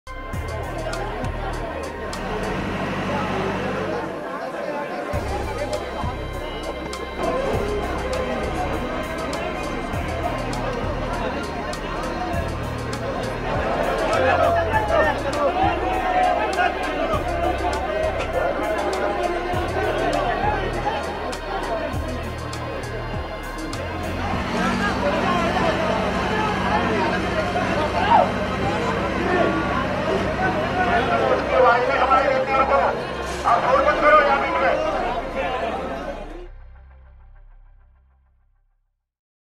5 Dead, 8 Rescued in Three-Storey Building Collapse in Ashok Vihar
One woman and four children were killed and at least seven injured after a three-storey building collapsed in northwest Delhi's Ashok Vihar on Wednesday, 26 September.